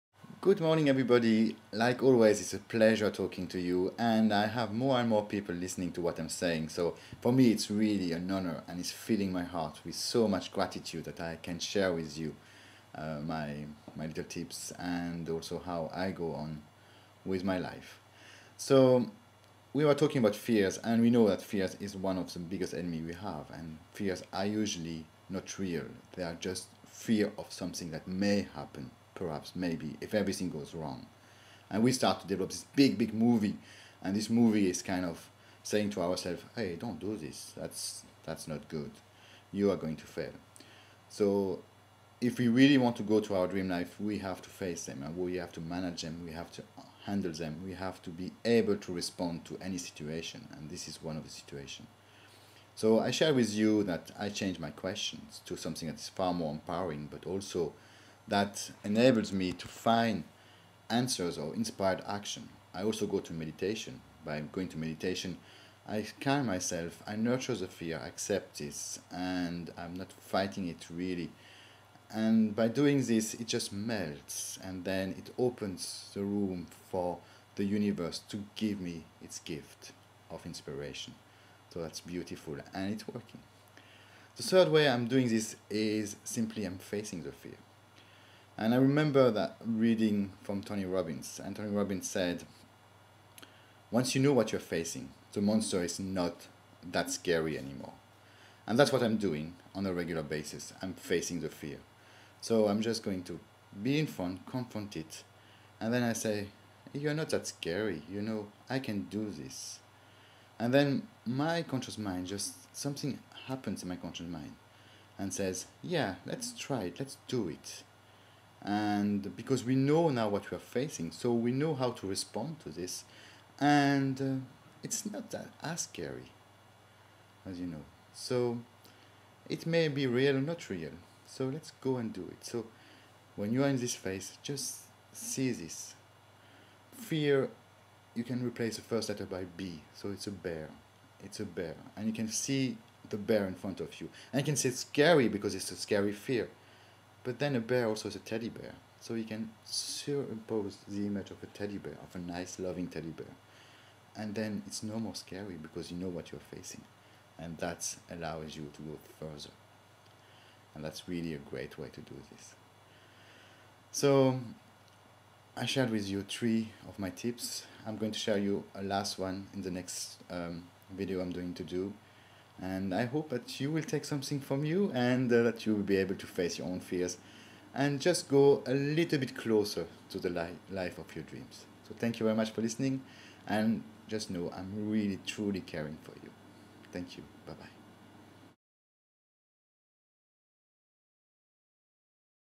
Master of Fears Part 3 - The Teddy Bear approach!
Fears...

They make us really roll out a movie in front of us, of what may happen to us in the future, and in the worse case, of how our lives will be affected if we do this 'thing'!  By this thing, I mean, "take an action", "Decide to do something", like go or pursue a relationship for example, or decide to become rich, and take the actions that will lead to this wealth!

So Fears are between us and the life of our dreams.

This is part 3 of my series about how to become master of your fears...

In fact Fear = Big Bear (just replace "F" by "B") ... that you can face... or transform in a nice Teddy Bear! Not so scary any more!

it is FREE!

Live without Fears, my Friend, and live your dreams!